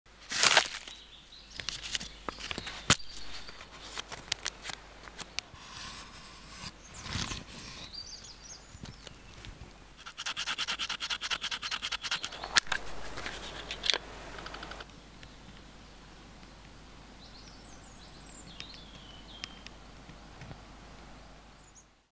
FIRE from foil and batteries🔥survival
You can kindle a fire using any current source and foil, which is always at hand

Hello 🫡
😉Donations for the development of my activities and paid messages! Thank you!